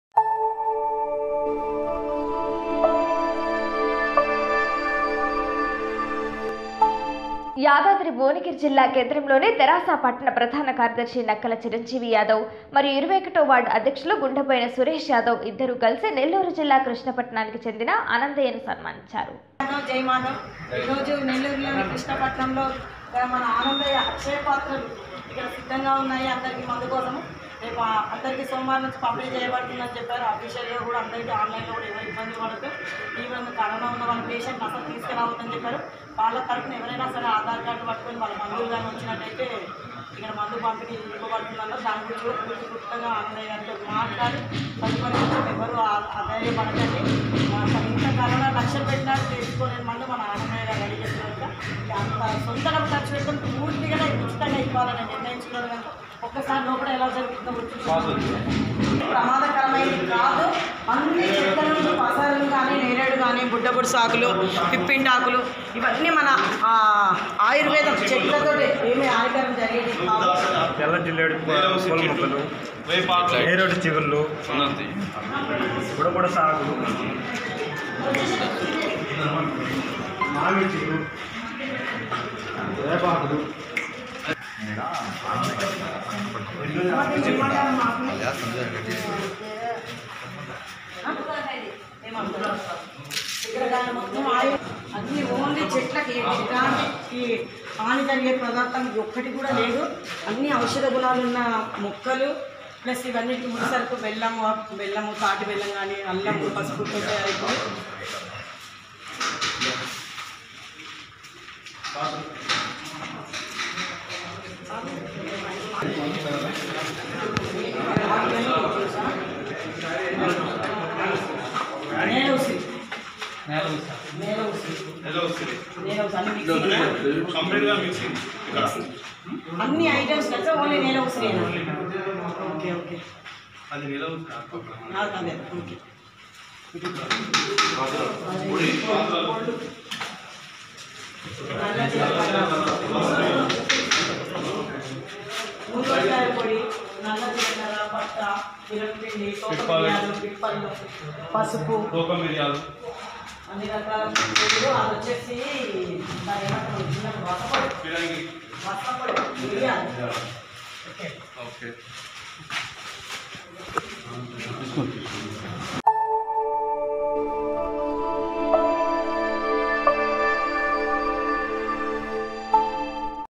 నెల్లూరు జిల్లా కృష్ణపట్నం కి చేరుకొని అనందయ్య యాదవ్ ను కలిసి సన్మానించడం జరిగినది
యాదాద్రి భువనగిరి జిల్లా భువనగిరి తెరాస పట్టణ ప్రధాన కార్యదర్శి నక్కల చిరంజీవి యాదవ్ మరియు 21వ వార్డ్ అధ్యక్షులు గుండెబోయిన సురేష్ యాదవ్ ఇద్దరు కలిసి నెల్లూరు జిల్లా కృష్ణపట్నం కి చేరుకొని అనందయ్య యాదవ్ ను కలిసి సన్మానించడం జరిగినది, మనలో చాలా మంది covid తోటి బాధ పడుతూ ఉన్నారు వారికోసం కూడా అక్కడ మాట్లాడడం జరిగింది, త్వరలో మన యాదాద్రిభువనగిరి జిల్లా కి అందరికీ అందుబాటులో మందును తీసుకుని వచ్చే దానికి మాట్లాడటం జరిగింది దానికి ఆనందయ్యా  స్పందించి తెలంగాణ లో మొదట ఉమ్మడి నల్లగొండ జిల్లా  తో మొదలు పెడుతామాన్నారు.